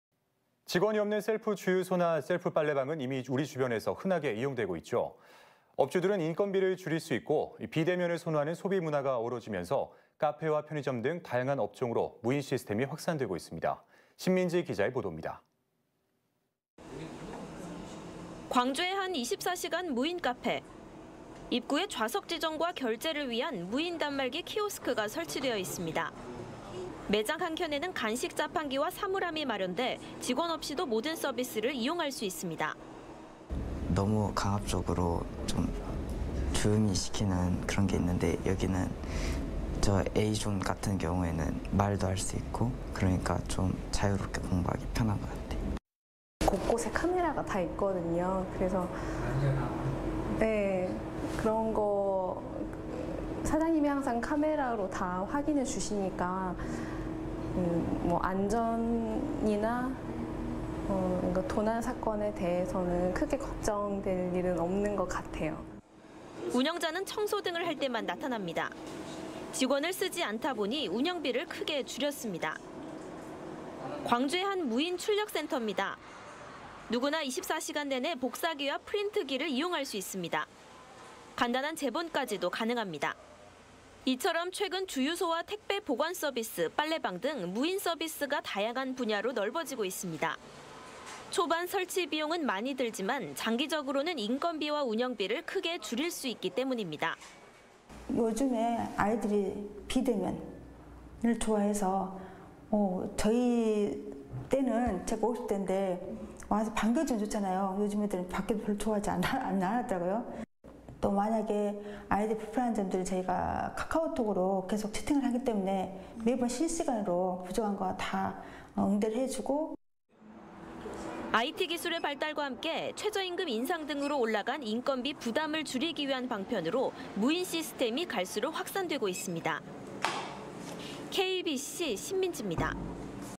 자동화 시대.. 무인 점포 '확산'
직원이 없는 셀프주유소나 셀프빨래방은 이미 우리 주변에서 흔하게 이용되고 있는데요.

업주들은 인건비를 줄일 수 있고, 비(非)대면을 선호하는 소비문화가 어우러지면서 카페와 편의점 등 다양한 업종으로 무인 시스템이 확산되고 있습니다.

신민지 기자의 보돕니다.